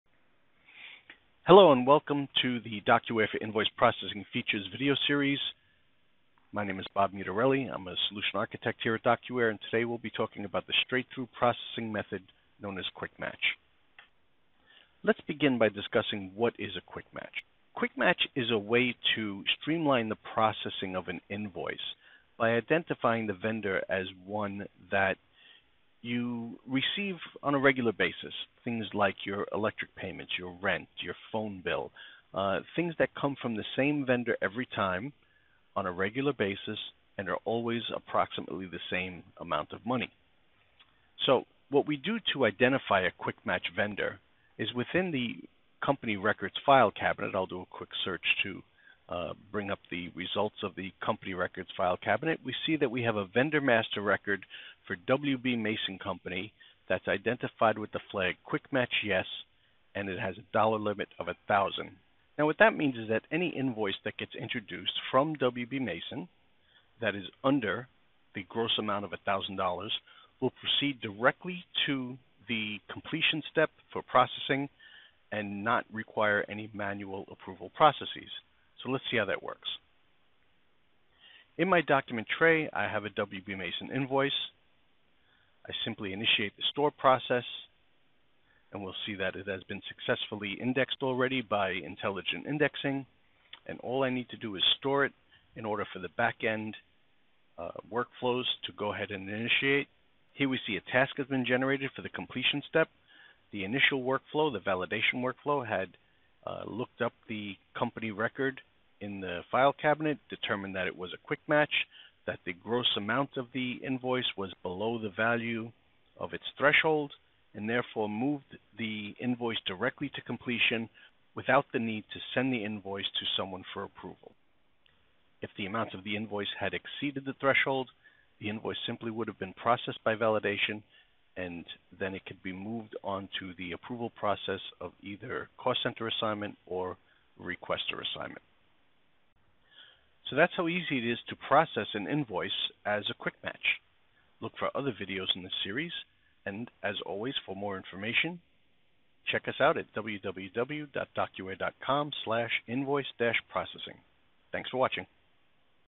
DocuWare for Invoice Processing Guided Tour 4: "QuickMatch”
See the straight through invoice process for recurring invoices including dollar thresholds using “QuickMatch”.